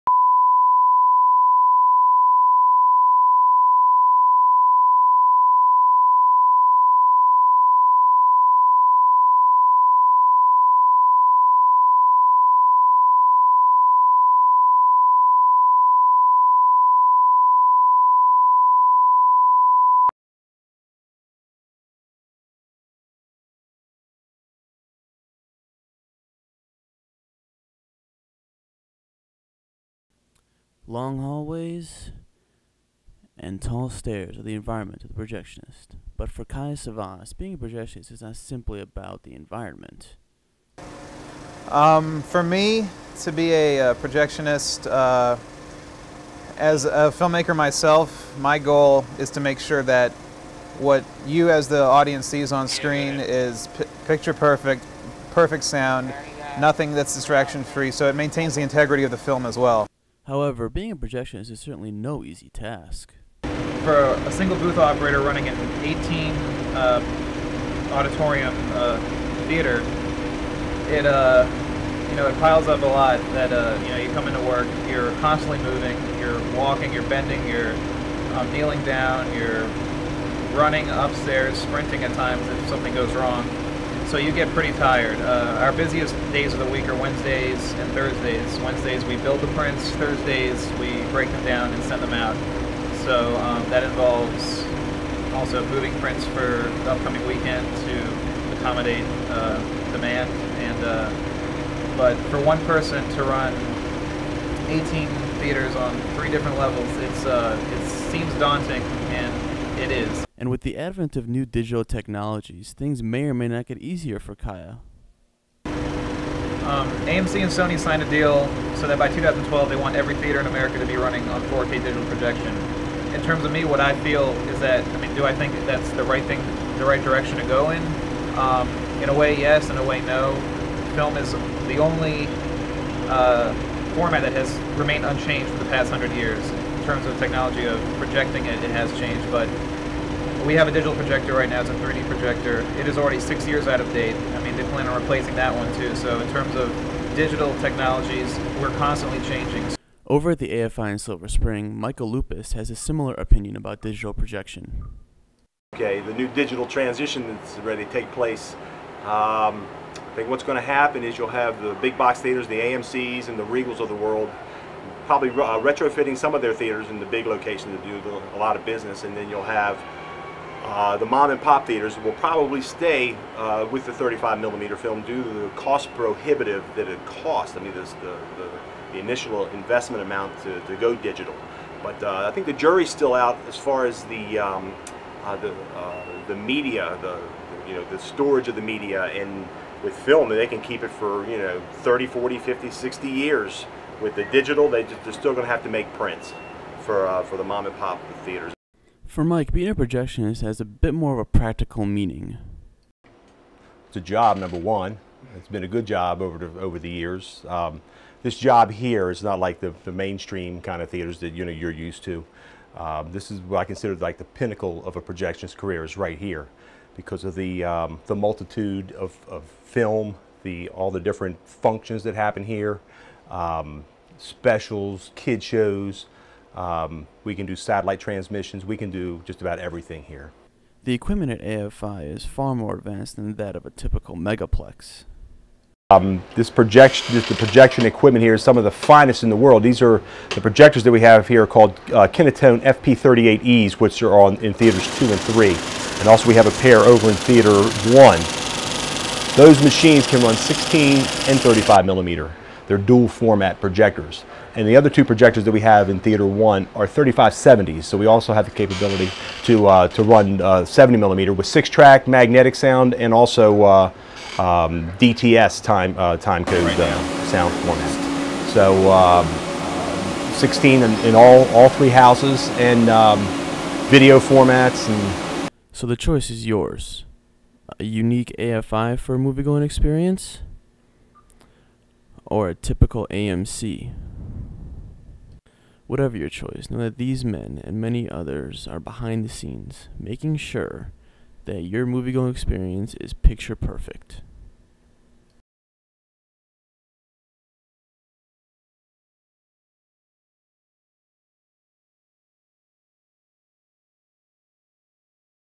AMC vs. AFI
Mini-documentary about the future of theatres, big and small...also featuring Kaya Savas.